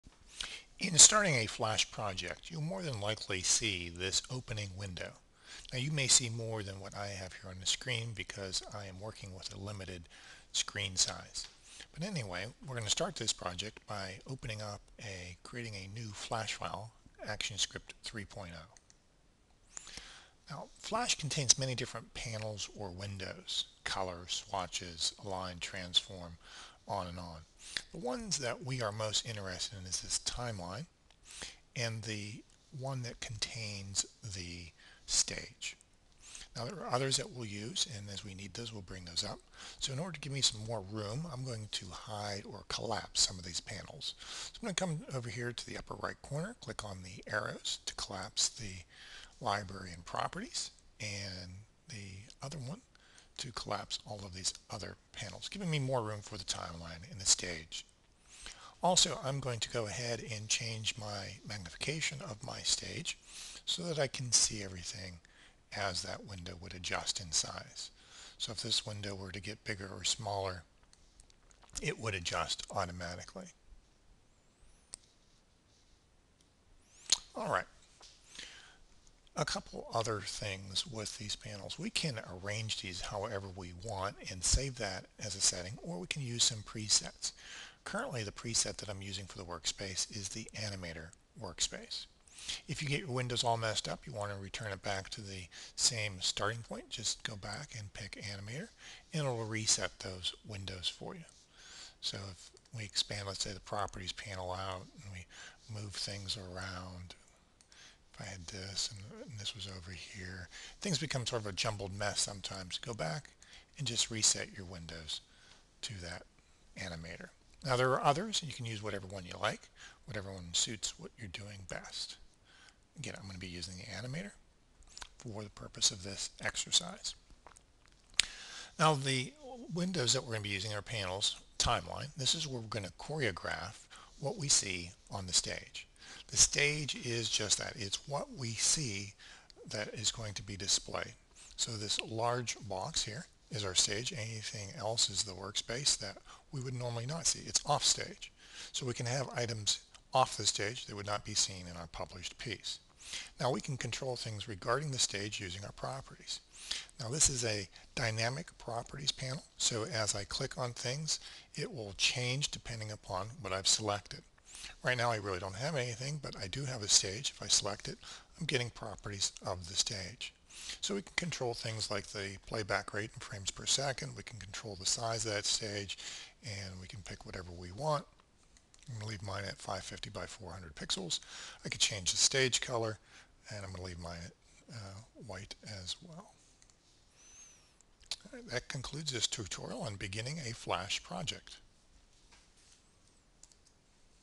Starting Flash
This video tutorial demonstrates how to start a Flash Project.  A brief explanation is given of the basic windows/panels and how to arrange them.